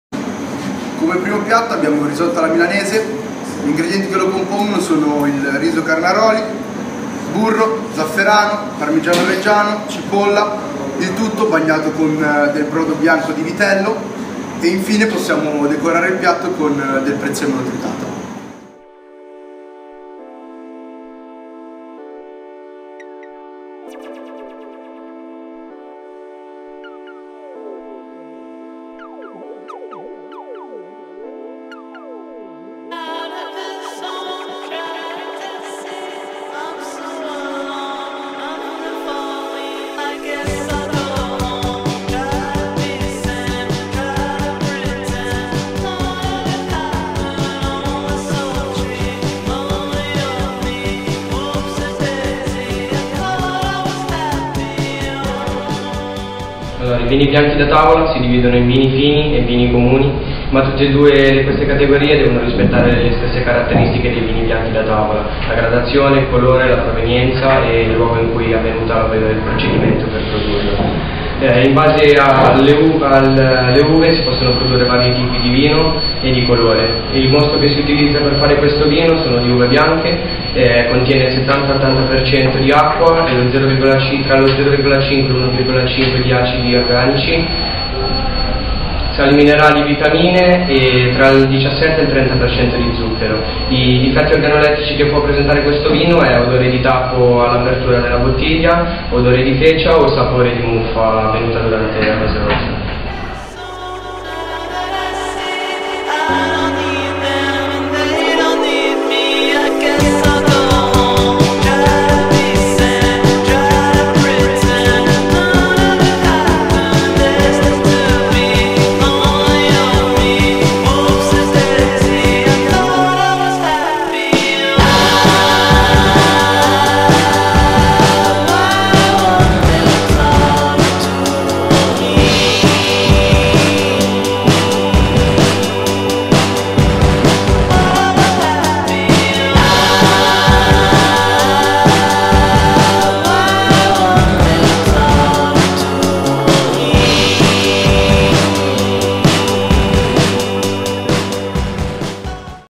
Risotto
Risotto alla milanese: video ricetta e conoscenza dei singoli ingredienti che compongono il risotto alla milanese, anche dal punto di vista nutrizionale, che compongono la pietanza.  3G-3I